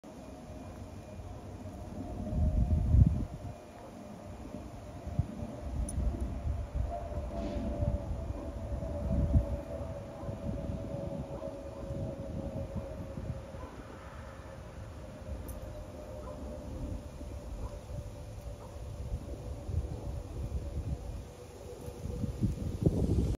Allegiant Airlines Airbus A320 - 11/7/22
Here is a video of an Allegiant Airlines Airbus A320 airplane in Manchester, Pennsylvania on Monday, November 7, 2022. Enjoy!